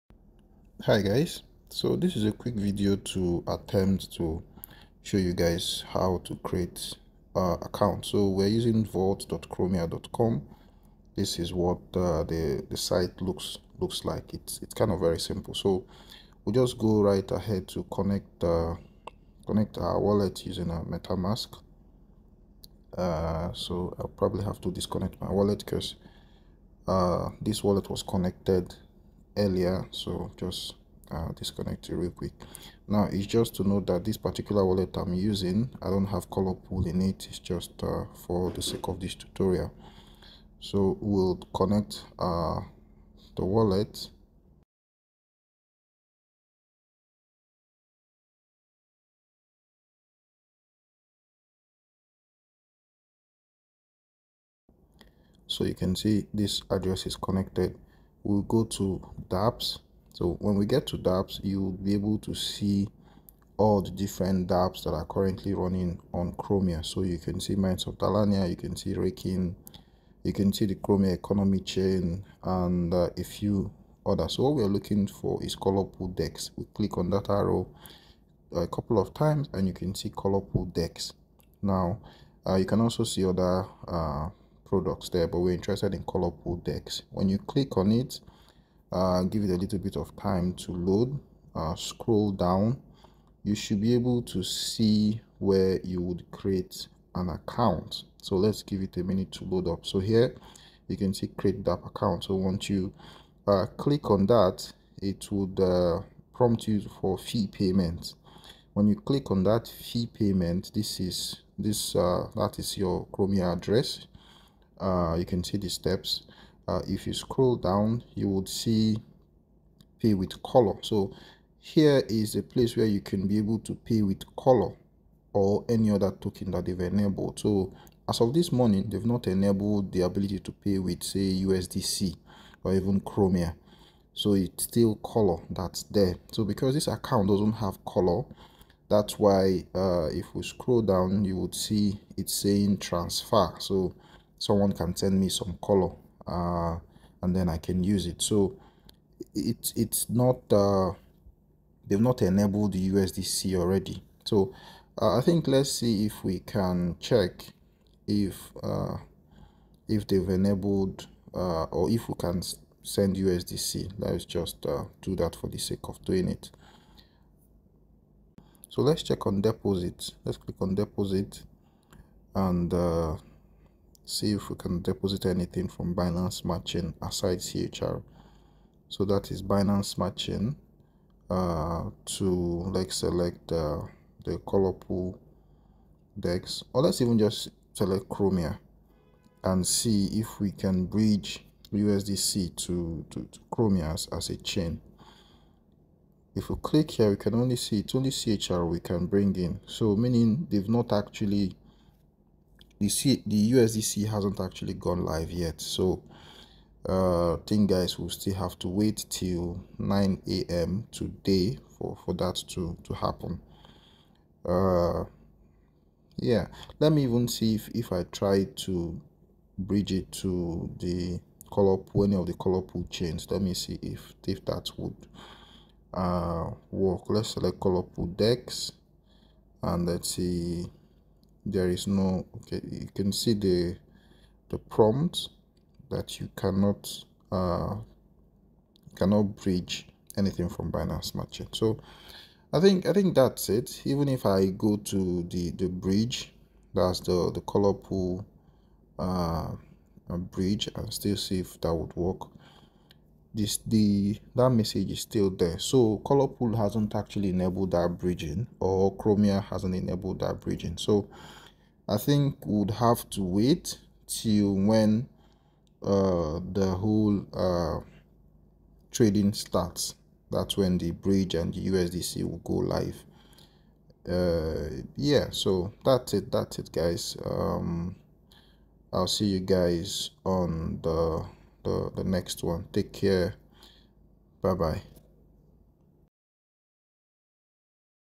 How to create a colorpool account on chromia
colorpool is chromias Premier dex and is launching today with its airdrop. This video tries to explain the process of creating an account in order to access the chain/dex.

Join the conversations here:
X: